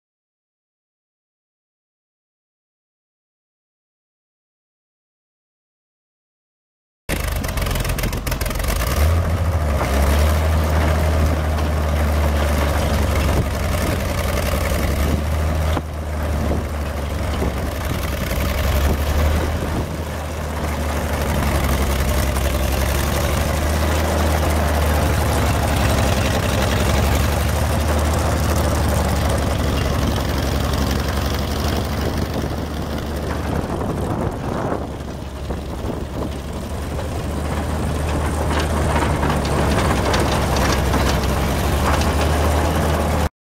Speicher Bros 600 tandem traction trencher for sale | sold at auction February 28, 2013
Speicher Bros 600 tandem traction trencher
6,732 hours on meter
GM 292 CID six cylinder gas engine
Six wheel drive
17"W cut to lay 10"W tile
6&#039; dig depth
12.4-24 tires
Lift cylinders have been rebuilt
Recently replaced teeth on wheel
Unknown winch working condition
Serial 65979

It was located in Burlington Junction, MO, and we sold it to the highest bidder for $9,460.